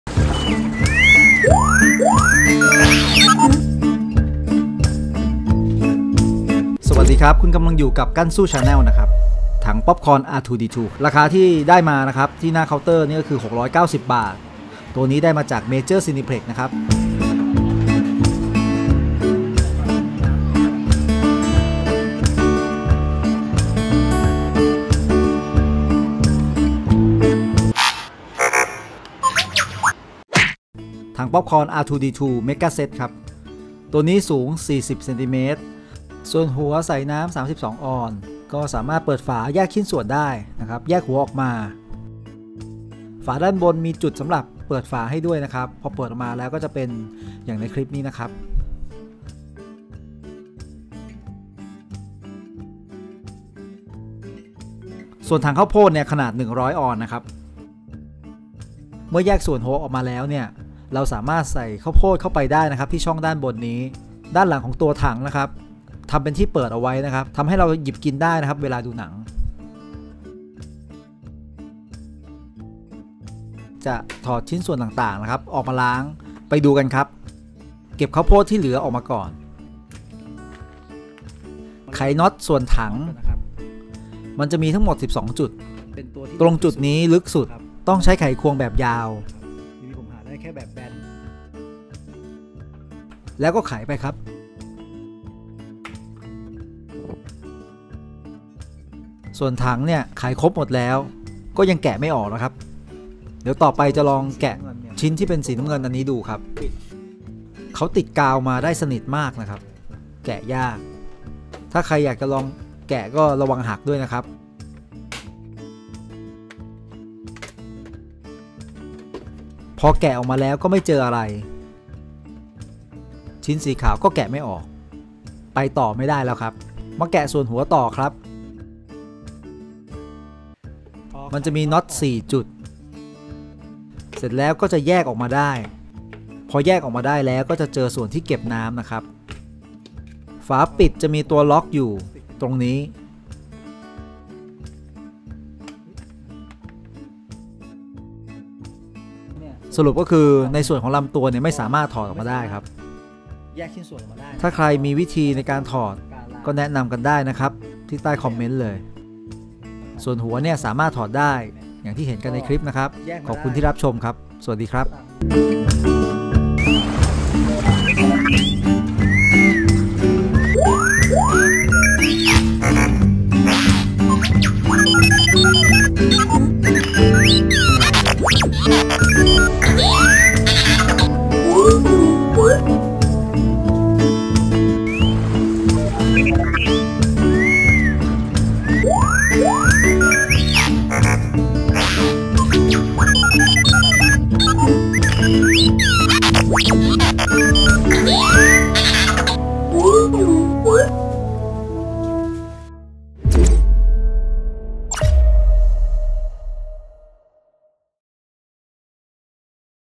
Review R2D2 Mega popcorn set [แยกชิ้นส่วนได้หรือไม่?]
Review R2D2 Mega popcorn set
 [แยกชิ้นส่วนได้หรือไม่?]
รุ่นนี้เป็นของเมเจอร์
ตั้งใจจะแยกชิ้นออกมา ล้างทำความสะอาด
แยกได้หรือไม่ไปดูกันครับ